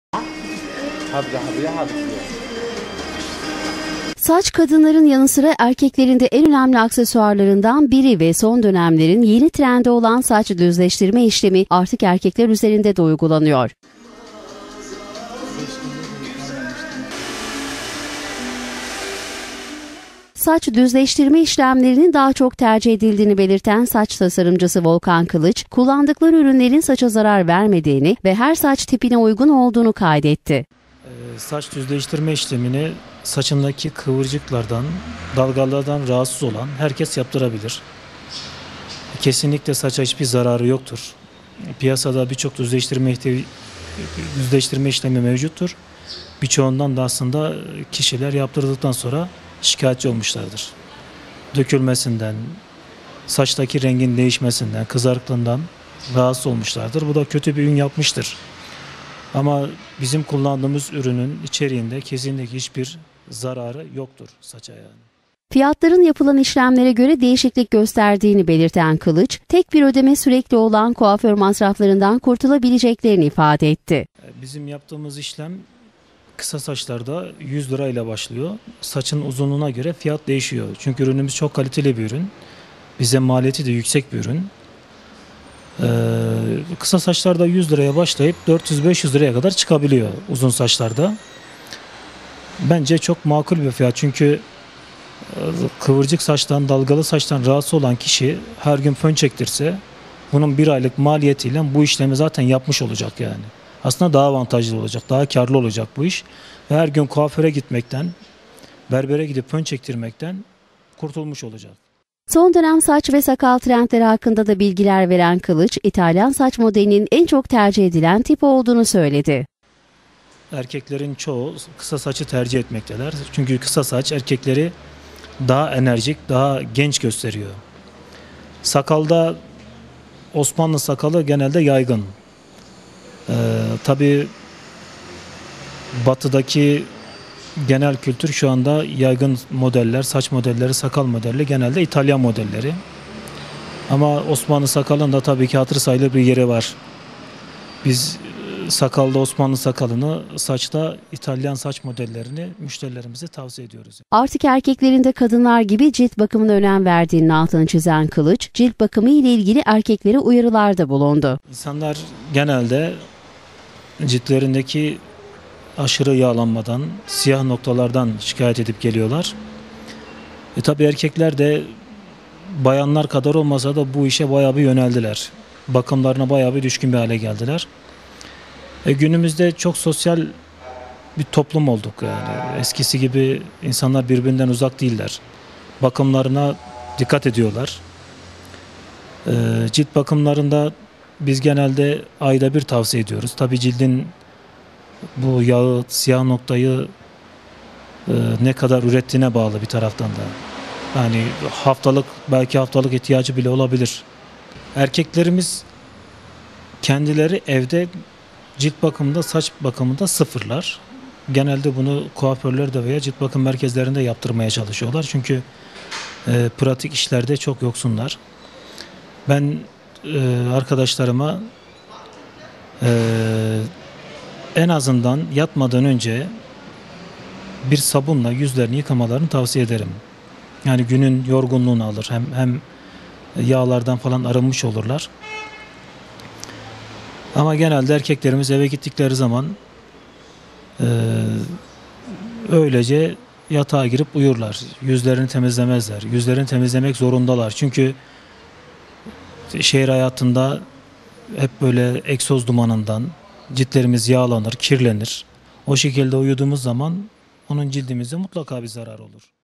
MALATYA'LI ERKEKLERİN YENİ TRENDİ SAÇ DÜZLEŞTİRME
Saç kadınların yanı sıra erkeklerinde en önemli aksesuarlarından biri ve Son dönemlerim yeni trendi olan saç düzleştirme işlemi artık erkekler üzerinde de uygulanıyor.